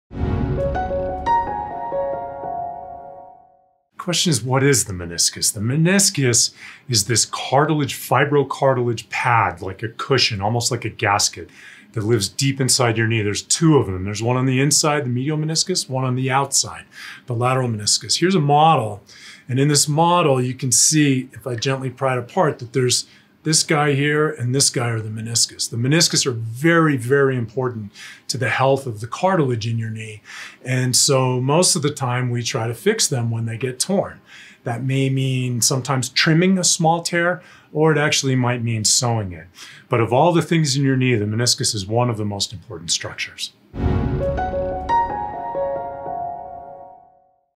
Orthopedic Surgeon: What is the Meniscus?
The Steadman Clinic's Thomas R. Hackett, MD describes the meniscus of the knee. for more information.